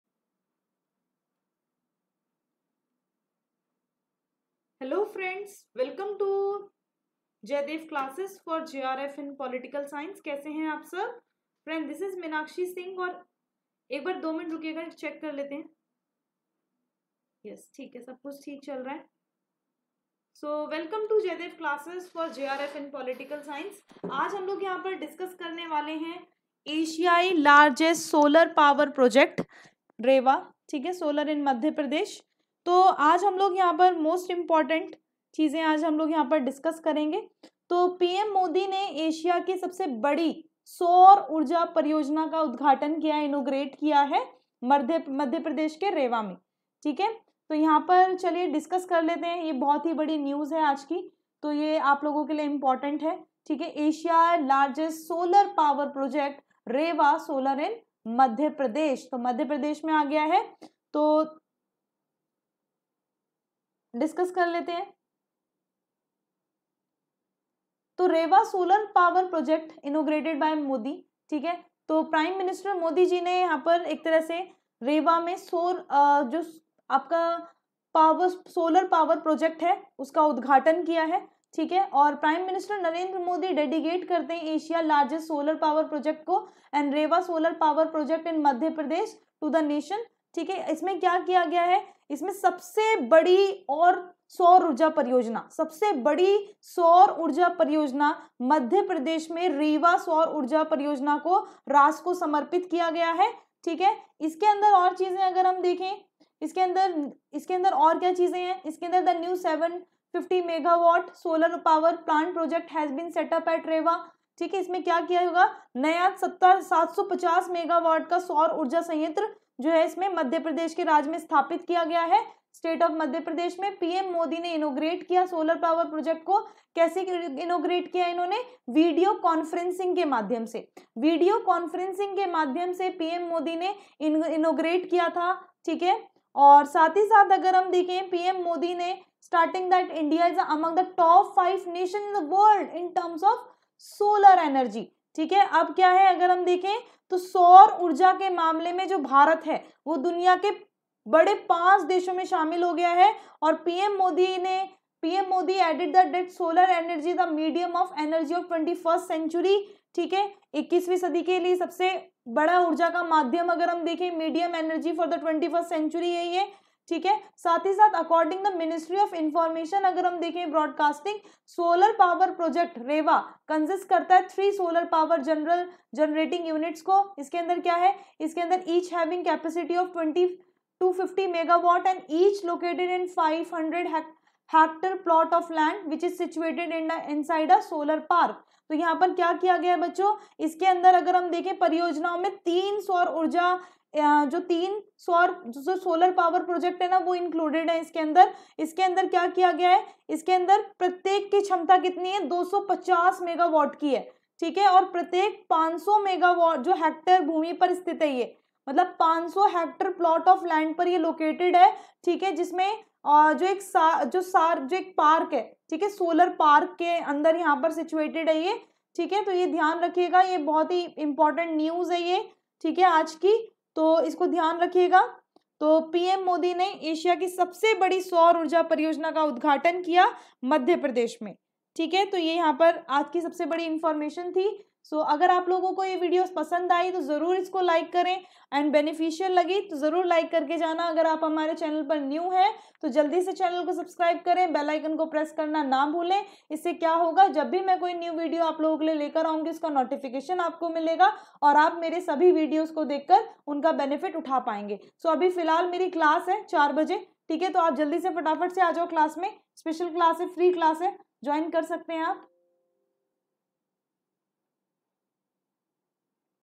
Asia's largest solar power project in Madhya Pradesh 2020
In this class, Meenakshi Singh will provide in-depth knowledge onAsia's largest solar power project in Madhya Pradesh 2020. The class will cover all concepts and would be helpful for aspirants preparing for NTA-UGC NET Exams. Learners at any stage of their preparation will be benefited by the Class.